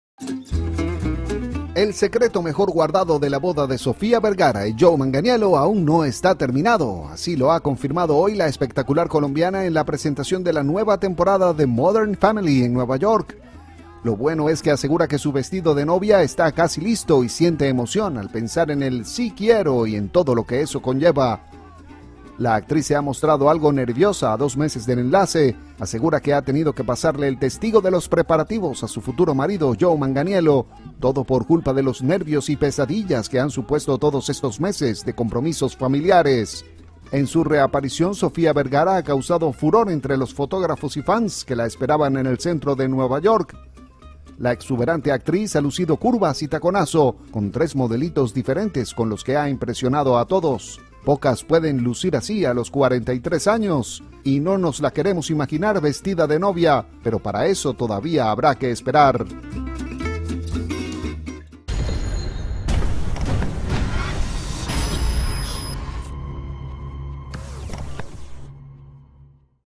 Sofía Vergara aún no tiene vestido de novia
El secreto mejor guardado de la boda de Sofía Vergara y Joe Manganiello aún no está terminado. Así lo ha confirmado hoy la espectacular colombiana en la presentación de la nueva temporada de "Modern Family" en Nueva York.
Lo bueno es que asegura que su vestido de novia está casi listo y siente emoción al pensar en el "sí quiero" y en todo lo que eso conlleva.
La actriz se ha mostrado algo nerviosa a dos meses del enlace. Asegura que ha tenido que pasarle el testigo de los preparativos a su futuro marido Joe Manganiello. Todo por culpa de los nervios y pesadillas que han supuesto todos estos meses de compromisos familiares.
En su reaparición Sofía Vergara ha causado furor entre los fotógrafos y  fans que la esperaban en el centro de Nueva York.
La exuberante actriz ha lucido curvas y taconazo con tres modelitos diferentes con los que ha impresionado a todos.
Pocas pueden lucir así, a los 43 años.
Y no nos la queremos imaginar vestida de novia. Pero para eso todavía habrá que esperar.